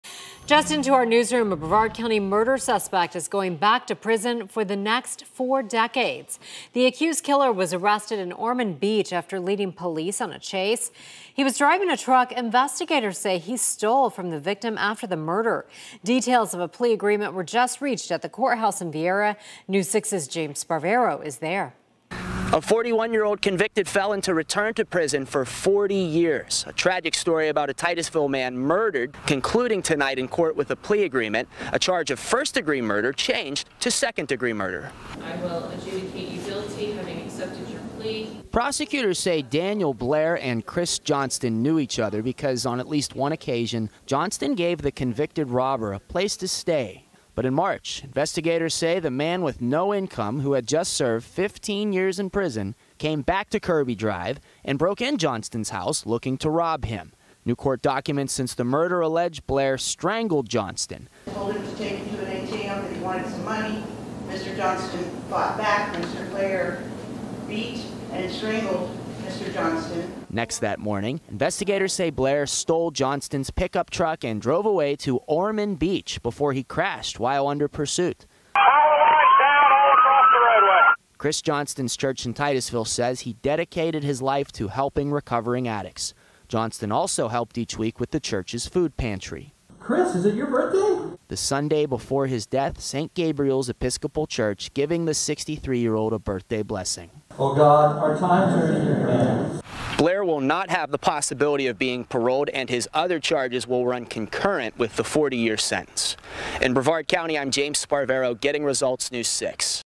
Man accepts plea deal for Brevard County murder
A convicted felon will serve 40 years in prison for killing a man.